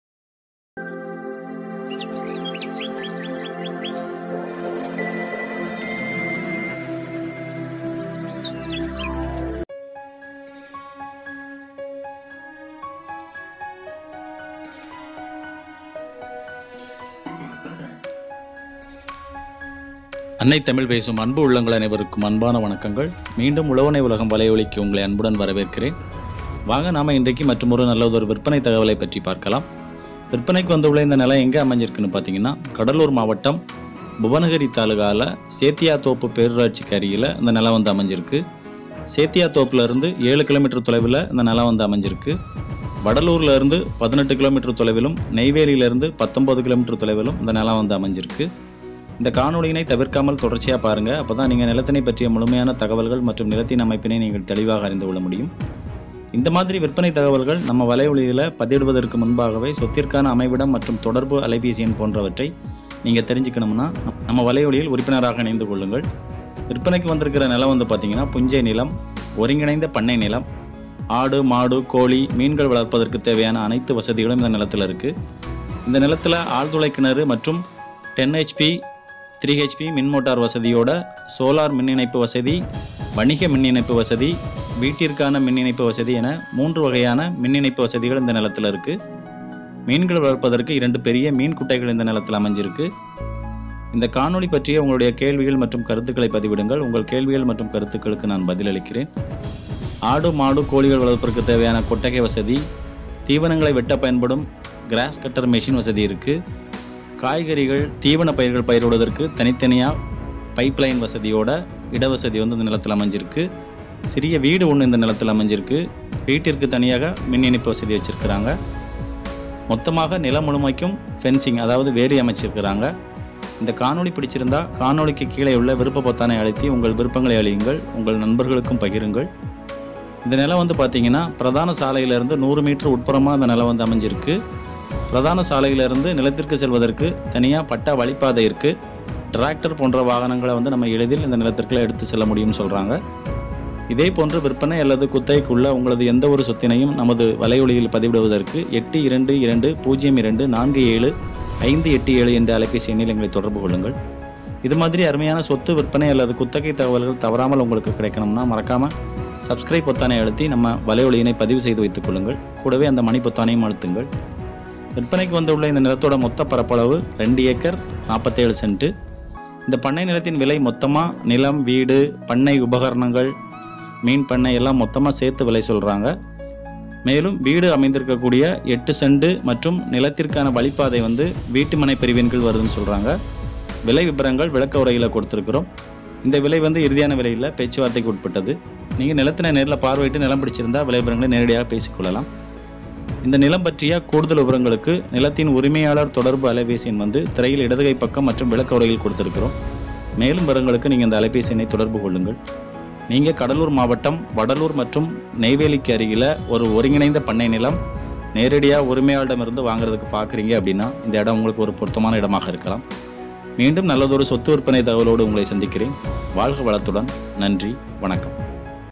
Owner Sale | வளமான 2.47 ஏக்கர் ஒருங்கிணைந்த பண்ணை நிலம் |Integrated farm land from owner|Tamil 1452
Owner Sale | வளமான 2.47  ஏக்கர் ஒருங்கிணைந்த பண்ணை நிலம் |Integrated farm land from owner| 1452

இந்த நிலம் எங்க அமைந்து இருக்குன்னு பாத்தீங்கன்னா கடலூர் மாவட்டம் புவனகிரி தாலுகாவுல சேத்தியாத்தோப்பு பேரூராட்சி அருகில இந்த நிலம் அமைந்திருக்கிறது.  சேத்தியாத்தோப்பில் இருந்து  7  km  தொலைவில் இந்த நிலம் அமைந்திருக்கிறது...வடலூரில் இருந்து 18km தொலைவில் இந்த நிலம் அமைஞ்சிருக்கு...நெய்வேலியில் இருந்து 19km தொலைவில் இந்த நிலம் அமைஞ்சிருக்கு

இந்த மாதிரி விற்பனை தகவல்கள் நம்ம வலையொளில் பதிவிடுவதற்கு முன்பாகவே சொத்திற்கான அமைவிடம் மற்றும் தொடர்பு எண் போன்றவற்றை நீங்க தெரிஞ்சிக்கனும் னா...நம்ம வலையொளியில் உறுப்பினராக இணைந்து கொள்ளுங்கள்

புஞ்சை நிலம்...ஒருங்கிணைந்த பண்ணை நிலம்...ஆடு மாடு கோழி மீன்கள் வளர்பதற்கு தேவையான அனைத்து வசதிகளும் இந்த நிலத்துல இருக்கு.

ஆழ்துளைக் கிணறு மற்றும் 10 hp 3 hp மின் மோட்டார் வசதியோட solar மின் இணைப்புவசதி இருக்கு...வணிக மின் இணைப்பு வசதி, வீட்டிற்கான மின் இணைப்பு வசதி என மூன்று வகையான மின் இணைப்பு வசதிகள் இந்த நிலத்துல இருக்கு...

மீன்கள் வளர்ப்பதற்கு இரண்டு பெரிய மீன் குட்டைகள் இந்த நிலத்துல அமைஞ்சிருக்கு

சாலையில் இருந்து 100m உட்புறமாக இந்த நிலம் அமைஞ்சிருக்கு. சாலையில் இருந்து நிலத்திற்கு செல்வதற்கு நல்ல அகலமான வழிப்பாதை உள்ளது

இந்த நிலத்தோட மொத்த  பரப்பளவு 2 ஏக்கர் 47 சென்ட்
இந்த நிலத்தின் விலை ஒரு,சென்ட் 45000 சொல்றாங்க. இந்த விலை வந்து இறுதியான விலை இல்லை...பேச்சுவார்த்தைக்குட்பட்டது.

வாழ்க வளத்துடன்...

நன்றி வணக்கம்...

👉  ID: 1452
📍 Location: Bhuvanagiri Taluk, Cuddalore
🚰 Water Source: Borewell
💡 Electricity: Solar EB, Commercial EB, Home EB
🛣️ Road Access: 100m from Tar Road
📐 Land Area: 2 Acres 47 Cents, Small House, 2 Fish Pond
💰 Price: Rs.45000 / Cent ( Slightly Negotiable)

Regards
Uzhavanae Ulagam
வாழ்க வளமுடன்...

நன்றி...
உழவனே உலகம்